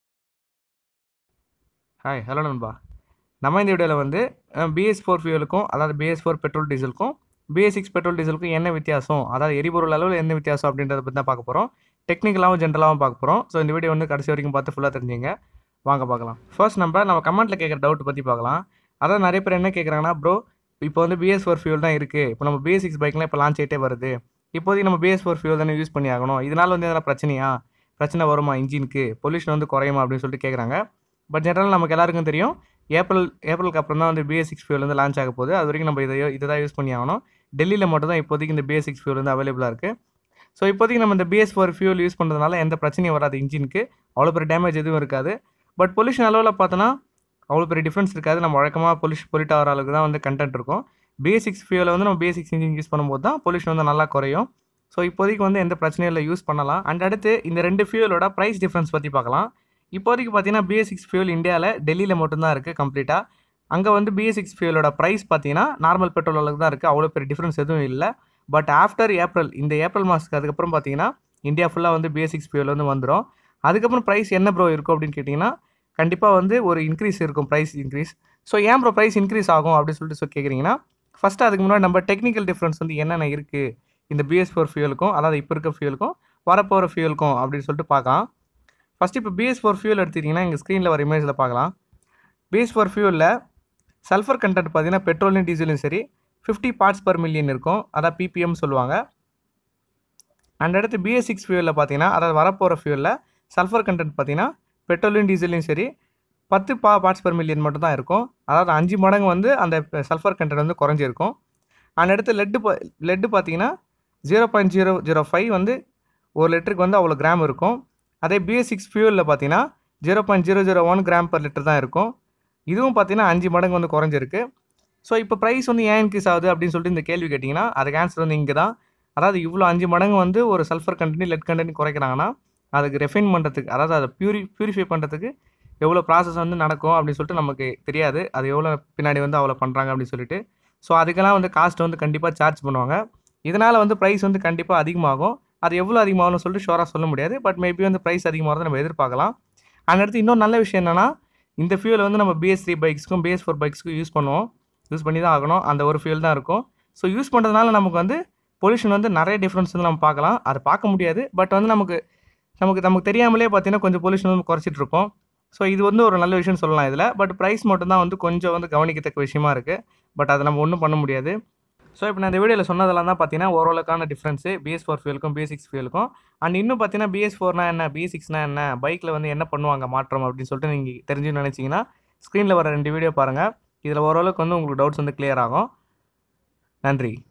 BS 4 Petrol vs BS 6 Petrol | இதில் என்ன வித்தியாசம்?? | Mech Edu Tamil.
In this video I explained about the comparison between the bs 4 fuel and bs 6 fuel in Tamil.